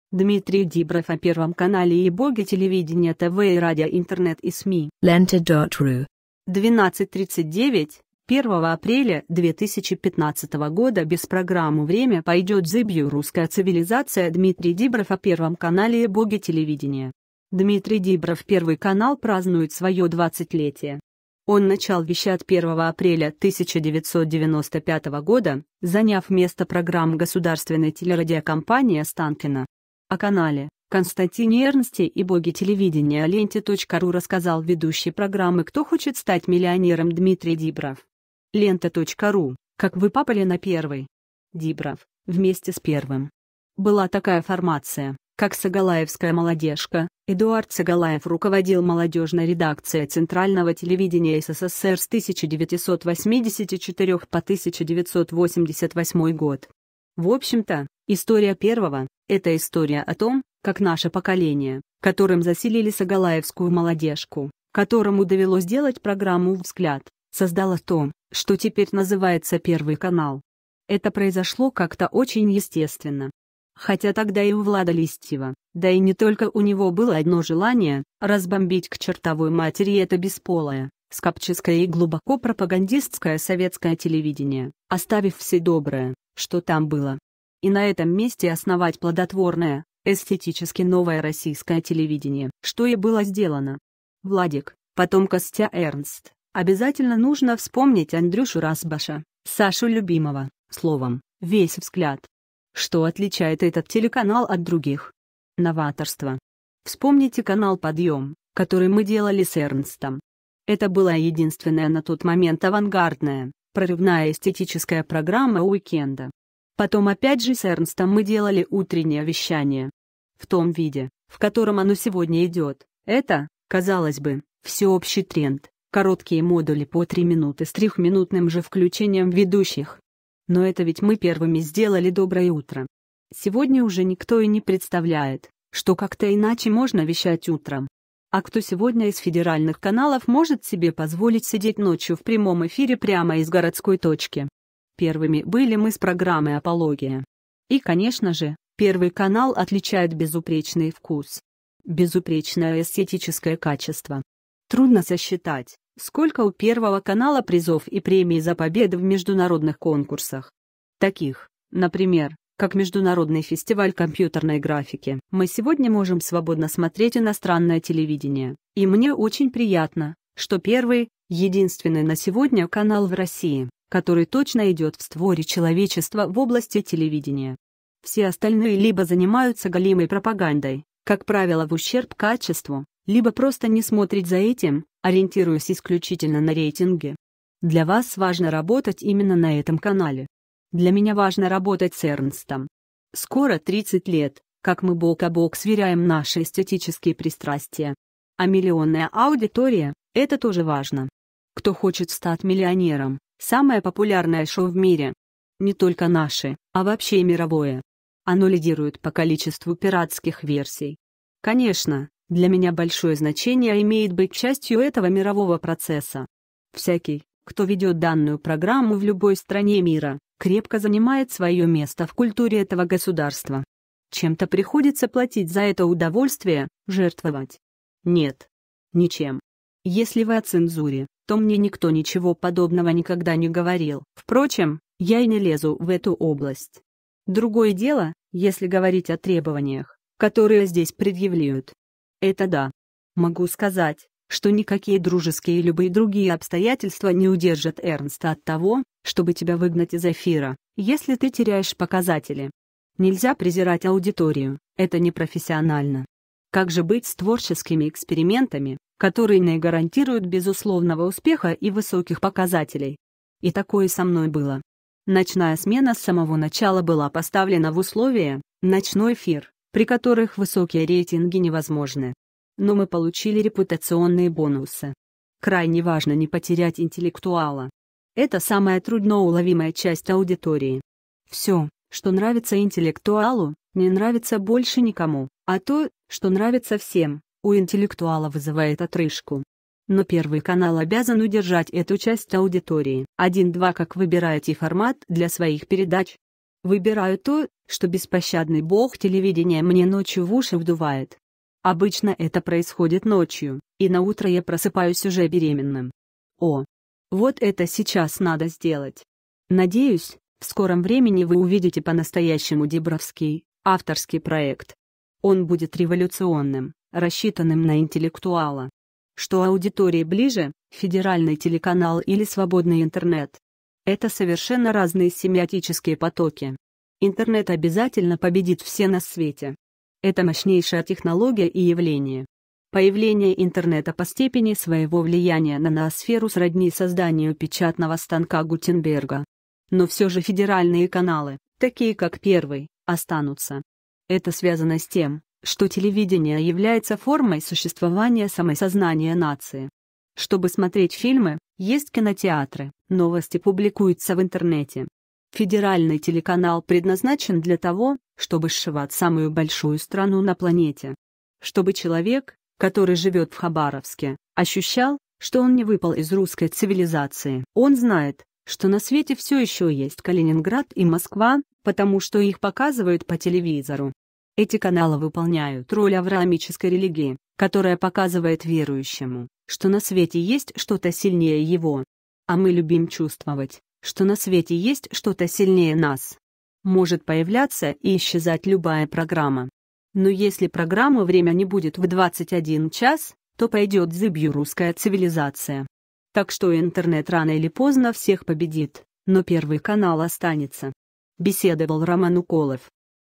LE150401 016 Дмитрий Дибров о Первом канале и боге телевид
Дмитрий Дибров о Первом канале и боге телевидения ТВ и радио Интернет и СМИ Lenta.ru 12:39, 1 апреля 2015 Без программы "Время" пойдет зыбью русская цивилизация Дмитрий Дибров о Первом канале и боге телевидения Дмитрий Дибров Первый канал празднует свое 20-летие. Он начал вещать 1 апреля 1995 года, заняв место программ государственной телерадиокомпании Останкино . О канале, Константине Эрнсте и боге телевидения Ленте.ру рассказал ведущий программы Кто хочет стать миллионером Дмитрий Дибров. Лента.ру : Как вы попали на Первый? Дибров: Вместе с Первым. Была такая формация, как сагалаевская молодежка (Эдуард Сагалаев руководил молодежной редакцией Центрального телевидения СССР с 1984 по 1988 год). В общем-то, история Первого это история о том, как наше поколение, которым заселили сагалаевскую молодежку, которому довелось делать программу Взгляд , создало то, что теперь называется Первый канал. Это произошло как-то очень естественно. Хотя тогда и у Влада Листьева, да и не только у него было одно желание разбомбить к чертовой матери это бесполое, скопческое и глубоко пропагандистское советское телевидение, оставив все доброе, что там было. И на этом месте основать плодотворное, эстетически новое российское телевидение. Что и было сделано. Владик, потом Костя Эрнст, обязательно нужно вспомнить Андрюшу Разбаша, Сашу Любимова словом, весь Взгляд . Что отличает этот телеканал от других? Новаторство. Вспомните канал Подъем , который мы делали с Эрнстом. Это была единственная на тот момент авангардная, прорывная эстетическая программа уик-энда. Потом опять же с Эрнстом мы делали утреннее вещание. В том виде, в котором оно сегодня идет, это, казалось бы, всеобщий тренд: короткие модули по три минуты с трехминутным же включением ведущих. Но это ведь мы первыми сделали Доброе утро . Сегодня уже никто и не представляет, что как-то иначе можно вещать утром. А кто сегодня из федеральных каналов может себе позволить сидеть ночью в прямом эфире прямо из городской точки. Первыми были мы с программой Апология . И конечно же, Первый канал отличает безупречный вкус. Безупречное эстетическое качество. Трудно сосчитать, сколько у Первого канала призов и премий за победы в международных конкурсах. Таких, например, как Международный фестиваль компьютерной графики. Мы сегодня можем свободно смотреть иностранное телевидение, и мне очень приятно, что Первый единственный на сегодня канал в России, который точно идет в створе человечества в области телевидения. Все остальные либо занимаются галимой пропагандой, как правило в ущерб качеству, либо просто не смотрят за этим, ориентируясь исключительно на рейтинги. Для вас важно работать именно на этом канале? Для меня важно работать с Эрнст